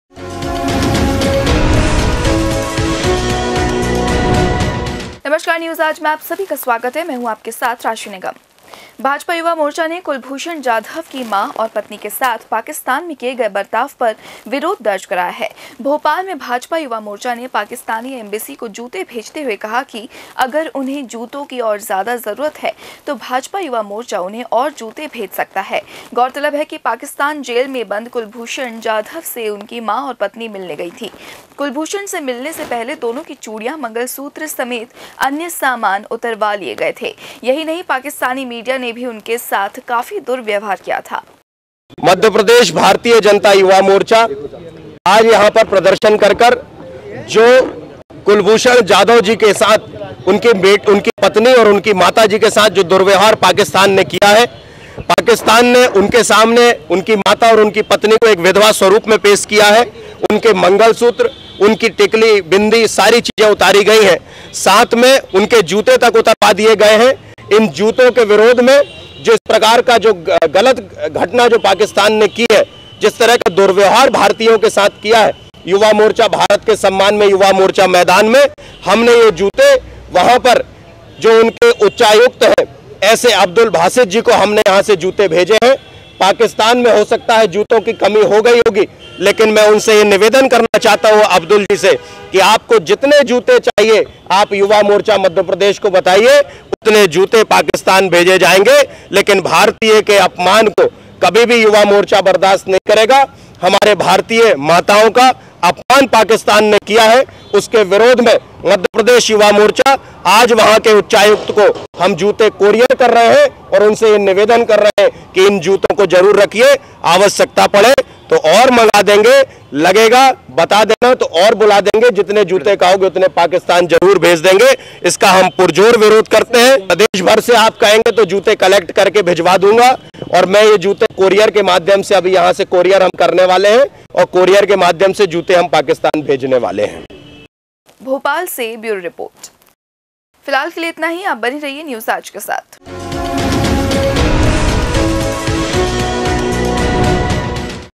भाजपा युवा मोर्चा ने पाकिस्तान एम्बेसी को भेजे जूते
कुलभूषण जाधव की मां और पत्नी के साथ किए व्यवहार पर विरोध दर्ज कराया